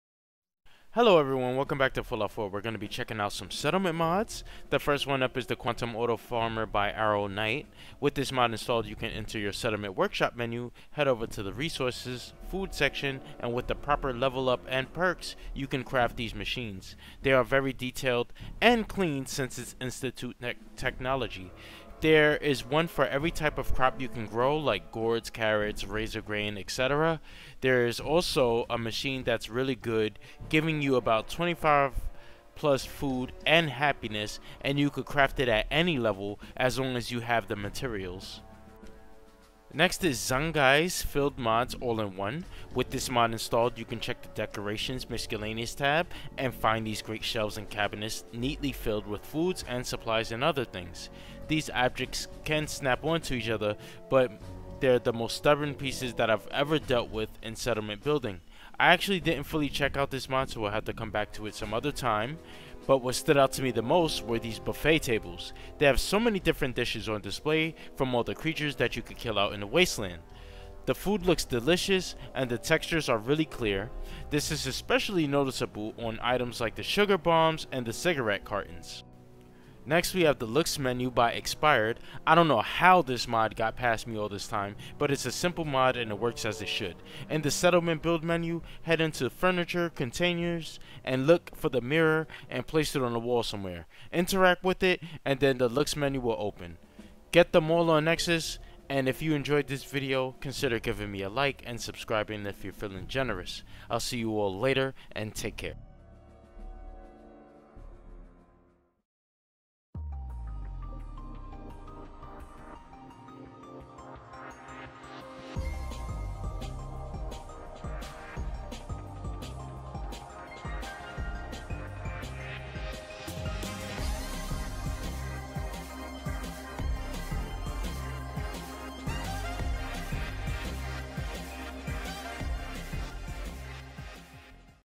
Institute Technology - The Quantum Auto Farmer
Here are some mods that I added to my load order, mostly for settlement building.
We'll go through a quick over view and let you know some things that maybe wrong with the mods or any incompatibilities with the current load order.